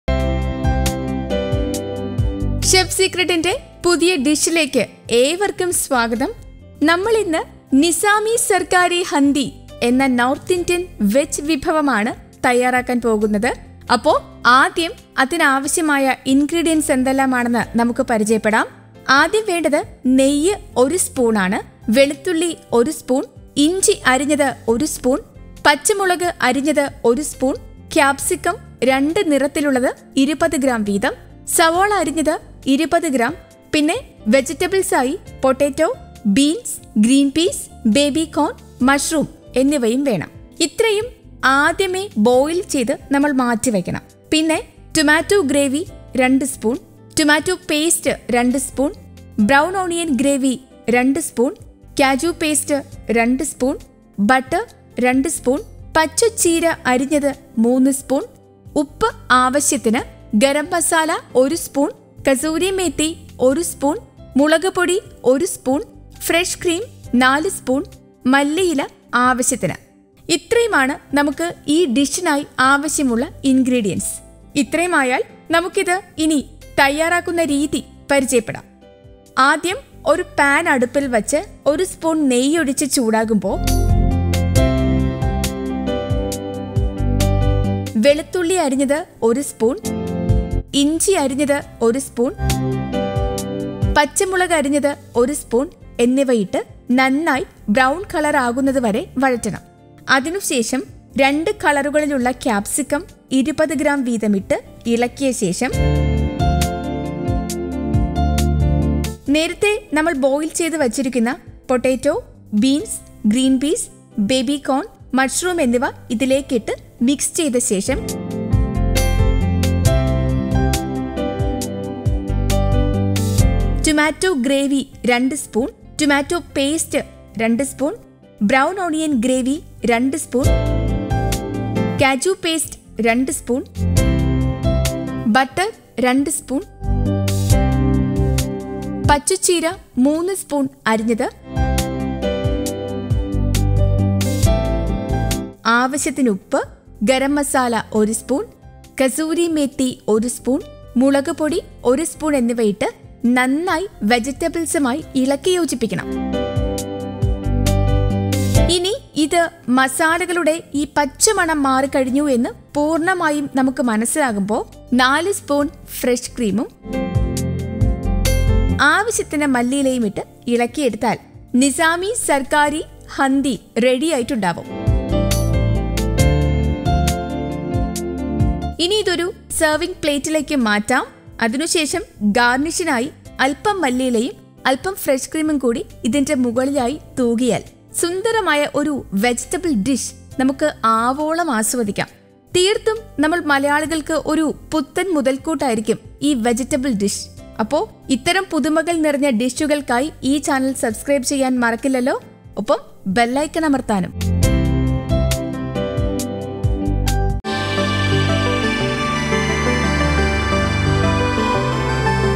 നോർത്ത് ഇന്ത്യൻ സ്പെഷ്യൽ വെജ് നിസാമി സർക്കാരി ഹന്തി | North Indian Special Veg Nizami Tarkari Handi
ഇൻഗ്രീഡിയന്റ്സ് :

നെയ്യ് - 1 സ്പൂൺ
വെളുത്തുള്ളി - 1 സ്പൂൺ
ഇഞ്ചി അരിഞ്ഞത് - 1 സ്പൂൺ
പച്ചമുളക് അരിഞ്ഞത് - 1 സ്പൂൺ
ക്യാപ്‌സിക്കം രണ്ട് നിറത്തിലുള്ളത് - 20 ഗ്രാം വീതം
സവാള അരിഞ്ഞത് - 20 ഗ്രാം
പൊട്ടറ്റോ, ബീൻസ്, ഗ്രീൻപീസ്, ബേബി കോൺ, മഷ്‌റൂം എന്നിവ
ടൊമാറ്റോ ഗ്രേവി - 2 സ്പൂൺ
ടൊമാറ്റോ പേസ്റ്റ് - 2 സ്പൂൺ
ബ്രൗൺ ഒണിയൻ ഗ്രേവി - 2 സ്പൂൺ
കാജു പേസ്റ്റ് - 2 സ്പൂൺ
ബട്ടർ - 2 സ്പൂൺ
പച്ച ചീര അരിഞ്ഞത് - 3 സ്പൂൺ
ഉപ്പ് ആവശ്യത്തിന്
ഗരം മസാല - 1 സ്പൂൺ
കസ്തൂരി മേത്തി - 1 സ്പൂൺ
മുളകുപൊടി - 1 സ്പൂൺ
ഫ്രഷ് ക്രീം - 4 സ്പൂൺ
മല്ലിയില ആവശ്യത്തിന്